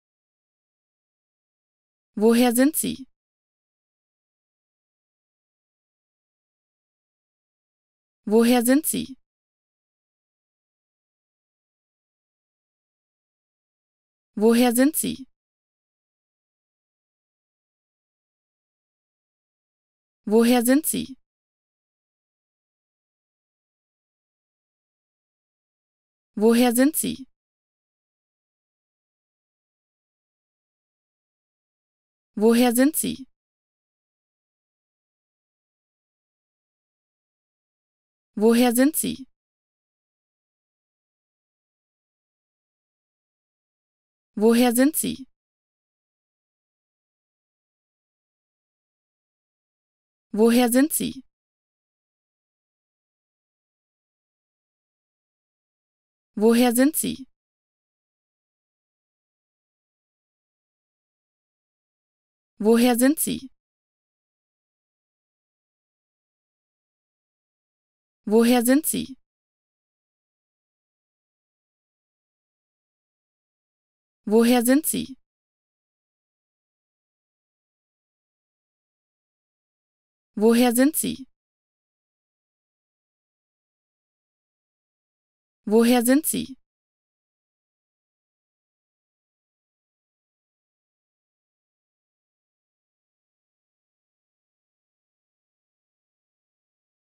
How to Pronounce ''Woher sind Sie'' (Where do you come from?) Correctly in German
Learn how to say ''Woher sind Sie'' in German with this short pronunciation tutorial.